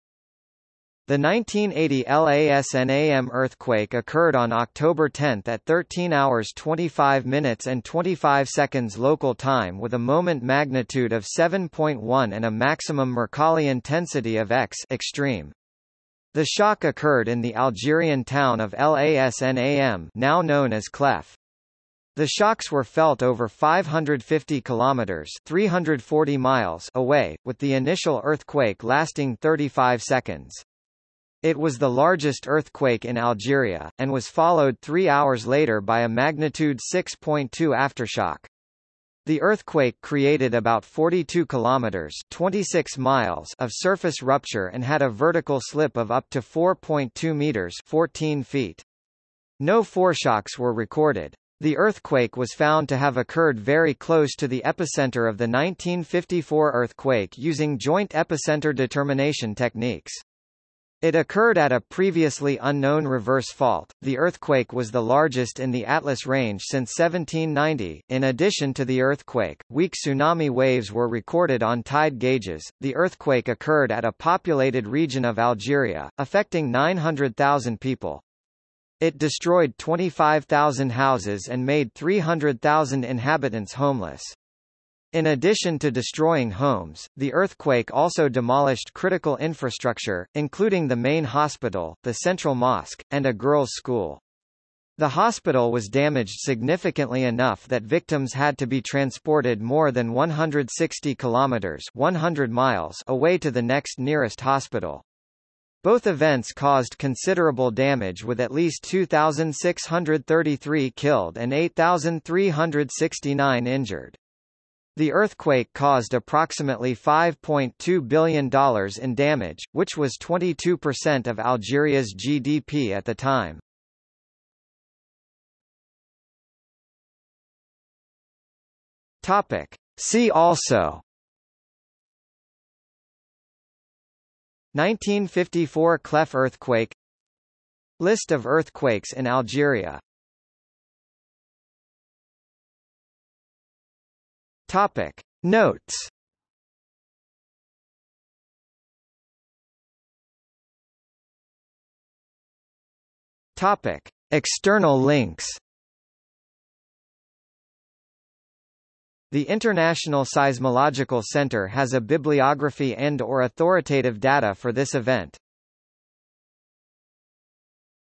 1980 El Asnam earthquake | Wikipedia audio article
00:02:05 See also
00:02:21 Notes
00:02:29 External links

- Socrates

SUMMARY
The 1980 El Asnam earthquake occurred on October 10 at 13:25:25 local time with a moment magnitude of 7.1 and a maximum Mercalli intensity of X (Extreme). The shock occurred in the Algerian town of El Asnam (now known as Chlef). The shocks were felt over 550 km (340 mi) away, with the initial earthquake lasting 35 seconds. It was the largest earthquake in Algeria, and was followed three hours later by a magnitude 6.2 aftershock. The earthquake created about 42 km (26 mi) of surface rupture and had a vertical slip of up to 4.2 m (14 ft). No foreshocks were recorded. The earthquake was found to have occurred very close to the epicenter of the 1954 earthquake using joint epicenter determination techniques. It occurred at a previously unknown reverse fault.The earthquake was the largest in the Atlas range since 1790.In addition to the earthquake, weak tsunami waves were recorded on tide gauges.The earthquake occurred at a populated region of Algeria, affecting 900,000 people. It destroyed 25,000 houses and made 300,000 inhabitants homeless. In addition to destroying homes, the earthquake also demolished critical infrastructure, including the main hospital, the central mosque, and a girls' school. The hospital was damaged significantly enough that victims had to be transported more than 160 km (100 mi) away to the next nearest hospital. Both events caused considerable damage with at least 2,633 killed and 8,369 injured. The earthquake caused approximately $5.2 billion in damage, which was 22% of Algeria's GDP at the time.